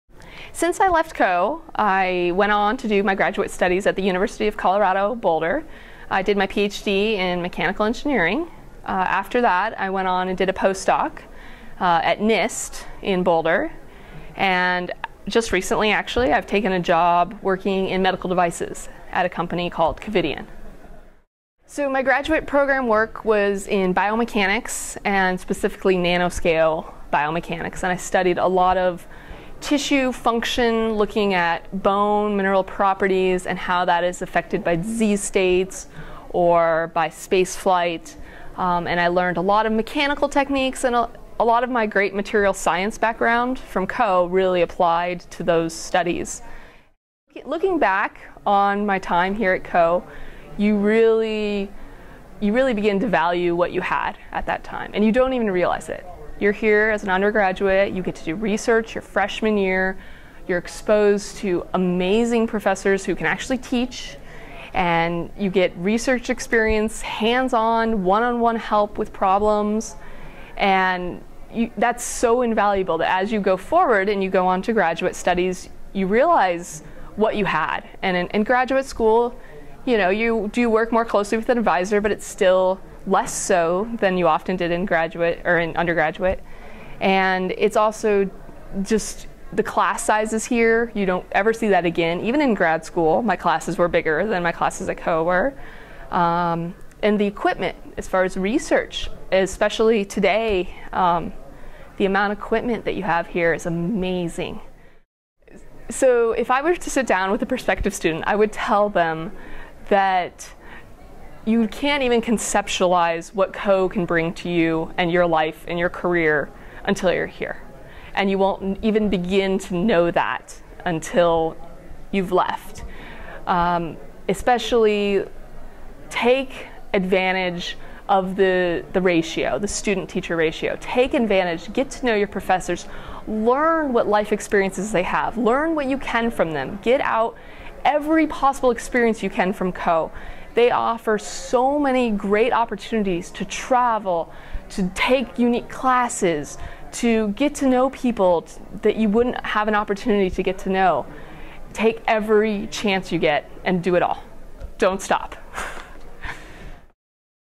Sara Campbell - Coe College Alumna
Sara Campbell '05 discusses life after graduation and her graduate program experience. Reflecting on her time at Coe, she also shares her advice for prospective students.  Sara graduated magna cum laude majoring in physics, mathematics and computer science at Coe. She went on to earn a Ph.D. in mechanical engineering at the University of Colorado - Boulder, which she specialized in biomechanics.  Sara has started her professional career working for a medical device company.